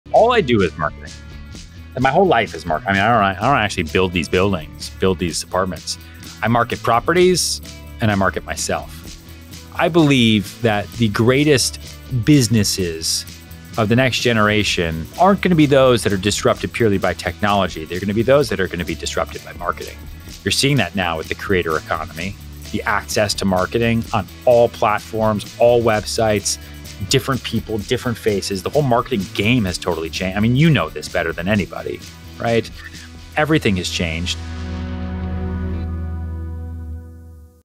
SERHANT. Founder Ryan Serhant | The Future is Going to be Disrupted by Marketing
Technology is in a constant state of evolution, so in the future, what’s going to truly capture attention and influence behavior is marketing and storytelling. Ryan Serhant, star of Million Dollar Listing: New York and founder of real estate giant SERHANT., doesn’t lead with tech in order to get attention. He leads his marketing with research that can be leveraged in publicity- making them thought leaders in the market as well as informed salespeople. Ryan's key piece of advice for those who are new to sales is to "take care of the work and the work will take care of you." If you pour your heart into your work, the work will reflect that and pay off for you in spades.

👇 Listen to the full episode on Spotify!